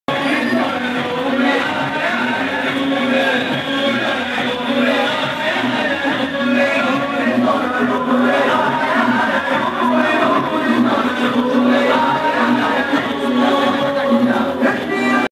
Eid milad un Nabi PBUH
Celebration of Eid milad un Nabi SAW at yakoot pura Hyderabad